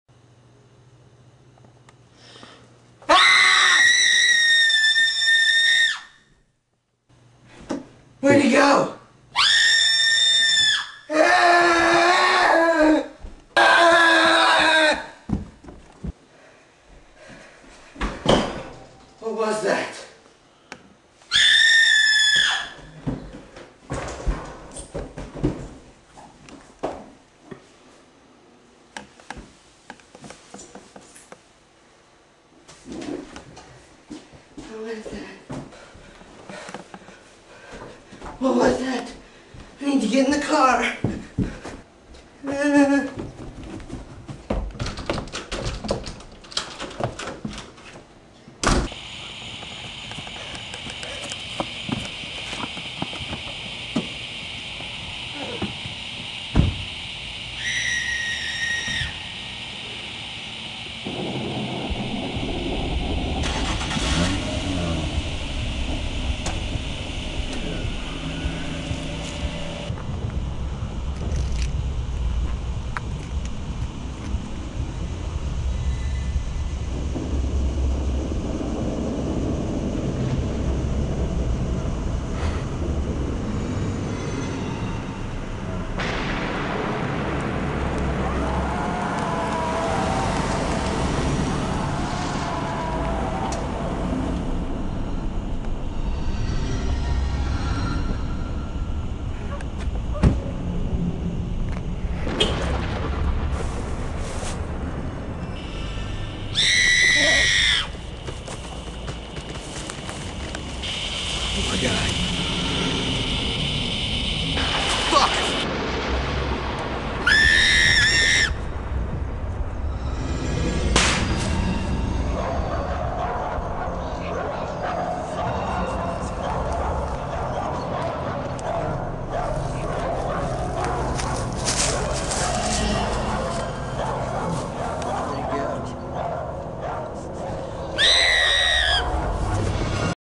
The Black Ghost
This movie has no plot to it. We were really bored and had a black trash bag.